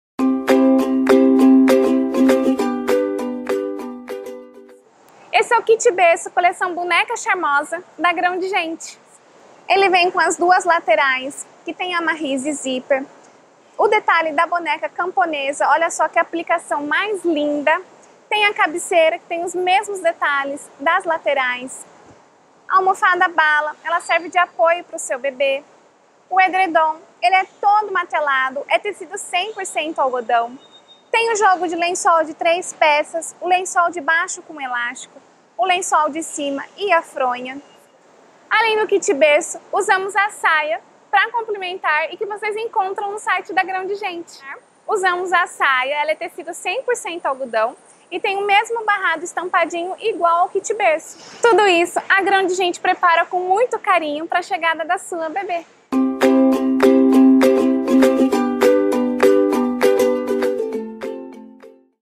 Kit Berço Boneca Charmosa | Ref: 47880
•1 Edredom de Berço 1,55m x 1,00m
•1 Cabeceira c/ Zíper 65cm x 38cm
•2  Laterais c/ Zíper 1,27m x 38cm
•1 Lençol de Cima 1,55m x 1,00m
•1 Lençol de Baixo c/ Elástico 1,55m x 1,00m
•1 Fronha 40cm x 30cm
•1 Almofada Apoio Bala 38cm x 14cm

Cor: Branco, Rosa e Floral